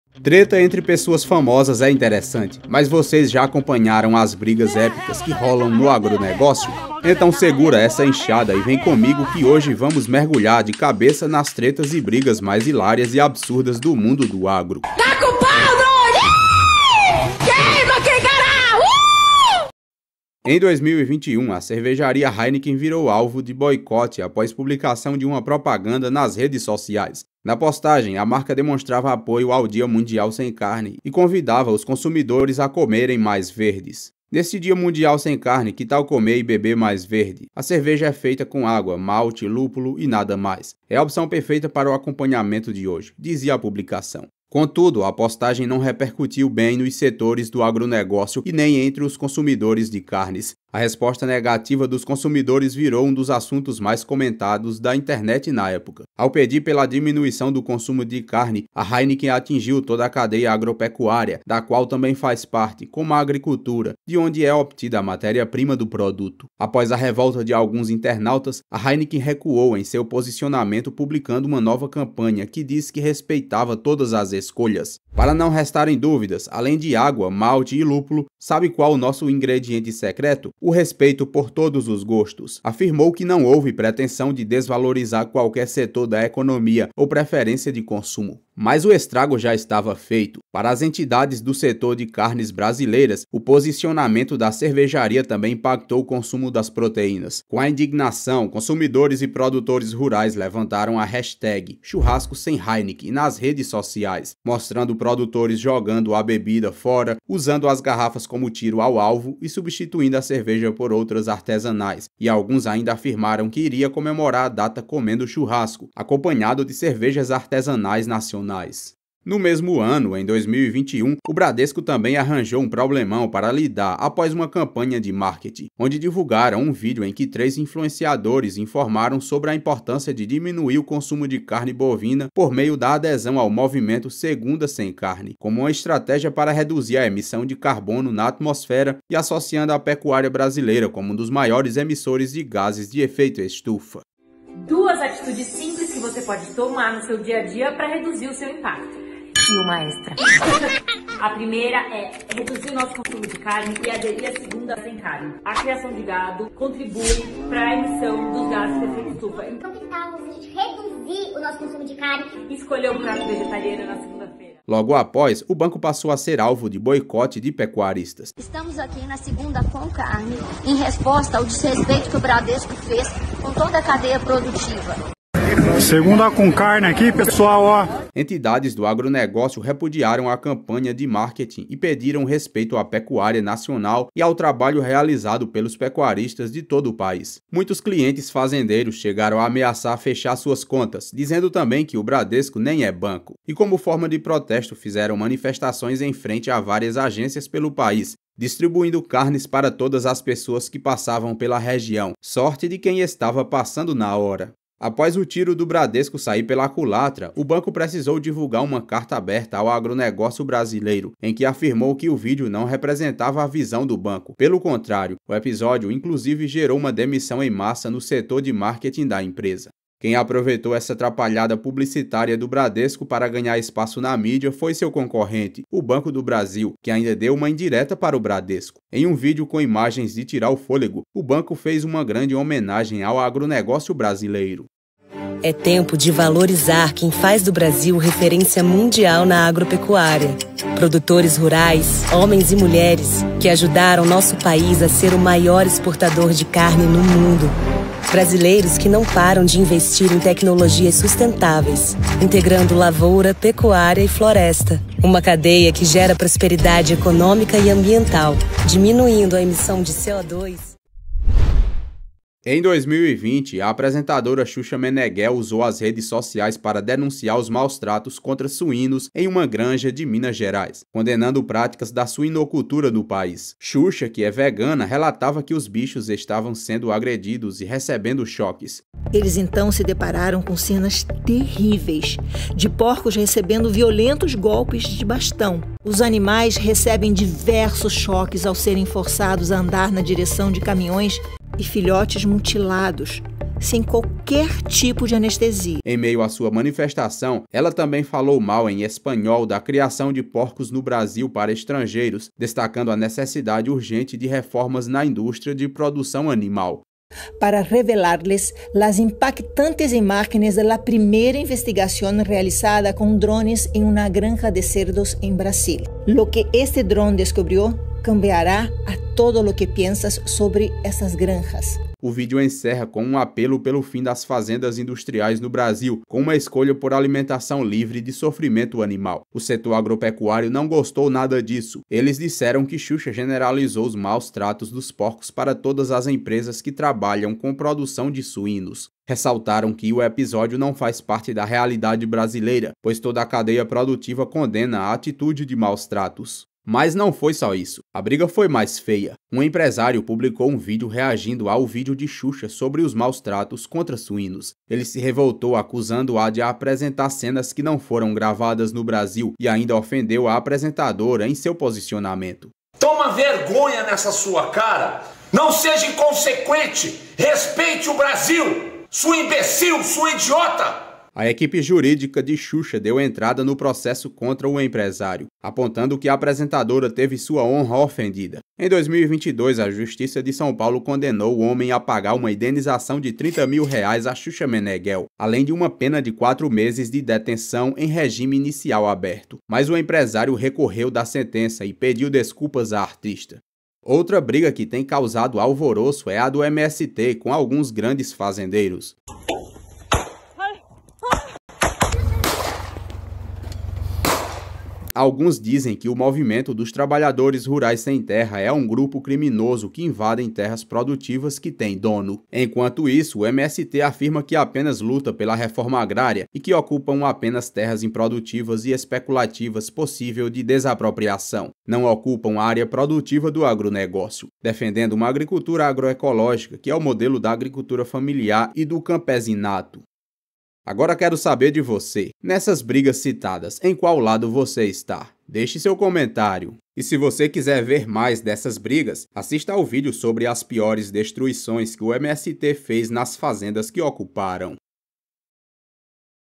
A MAIOR BRIGA DO AGRONEGÓCIO
Vocês já acompanharam as brigas épicas que rolam no agronegócio? Confira as tretas e brigas mais hilárias e absurdas do mundo do agro!